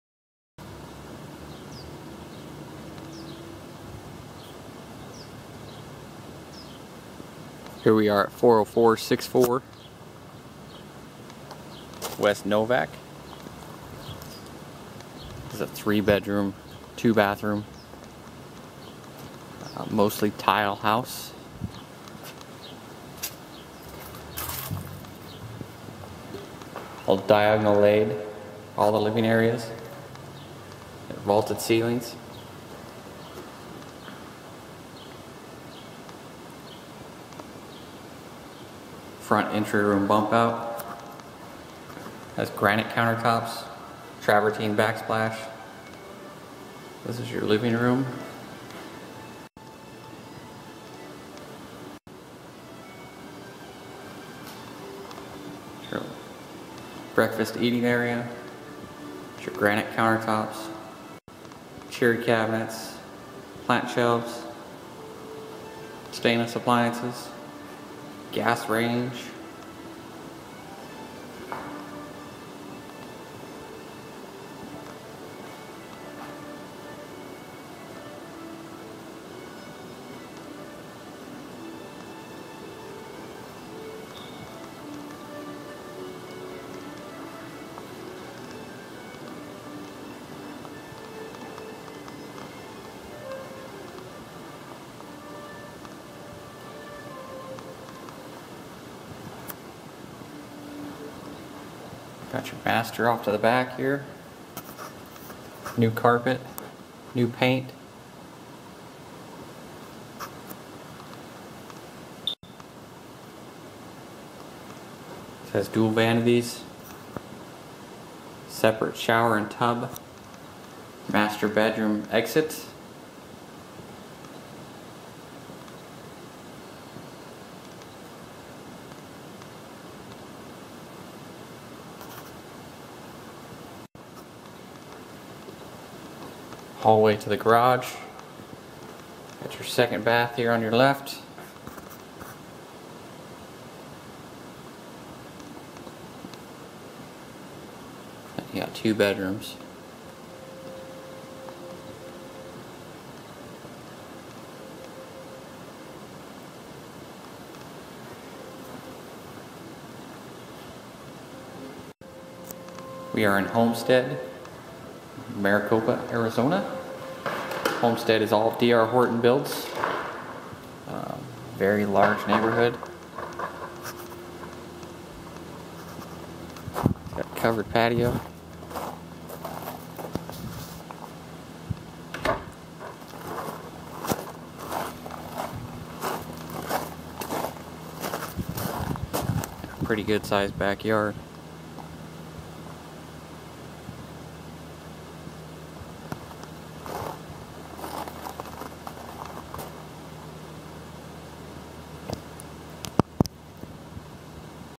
40464 W NOVAK LN Maricopa, AZ 85138, Listed by MaricopaPro.com
MaricopaPro.com...  BETTER THAN NEW stunning REMODEL with the BEST of EVERYTHING! CHERRY cabinets! RARE Verde Butterfly Granite! AWESOME huge DIAGONAL tile EVERYWHERE except the bedrooms! NEW PREMIUM CARPET and two-tone DECORATOR PAINT! CATHEDRAL ceilings THROUGHOUT with ARCHITECTURAL ARCHED WINDOWS and DOORWAYS! PREMIUM DOORS and HARDWARE!Oil-rubbed BRONZE fixtures! CHEF's granite ISLAND kitchen features TUSCAN TRAVERTINE BACKSPLASH, STAINLESS STEEL appliance package with GAS STOVE and MASSIVE WALK-IN PANTRY! MASTER SUITE has HUGE WALK-IN CLOSET, DUAL MARBLE VANITY, STEP-IN SHOWER, SEPARATE SOAKING TUB with MARBLE SURROUND, and PRIVATE DOOR TO BACKYARD PATIO! Huge POOL-SIZED backyard is graveled and ready to go! GAS heat with NEW GAS WATER HEATER! EPOXY-FLOORED garage with new PREMIUM INSULATED GARAGE DOOR! Listed by Tony Schumacher, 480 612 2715  .. Call for your home listed here!!